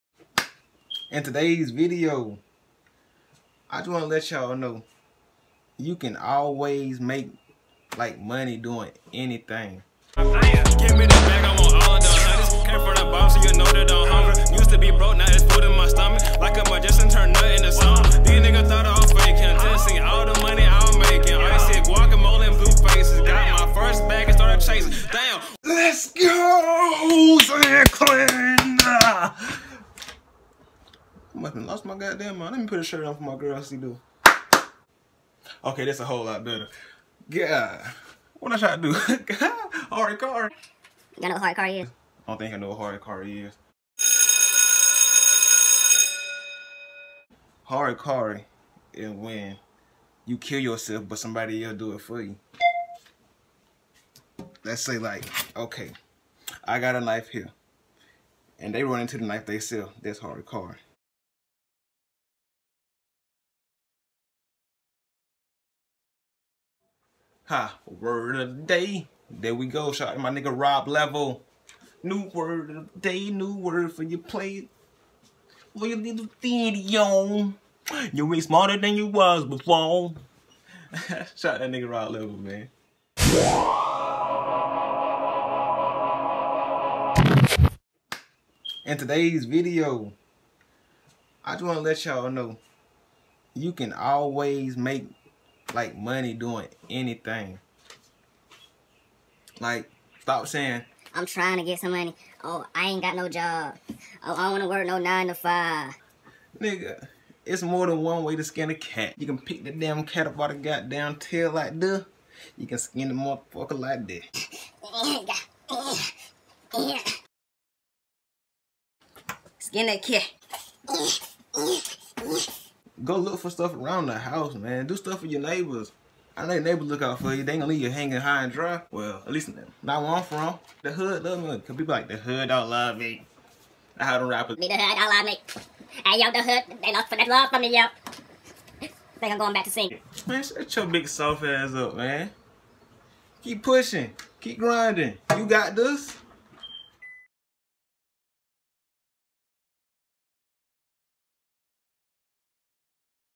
Make Money Doing Anything... LITERALLY
In Todays Video I Demonstrate How You Can Make Money Doing Anything Literally...

Leave A Comment On What You Liked Or What I Can Change

Shoutout To Rob Level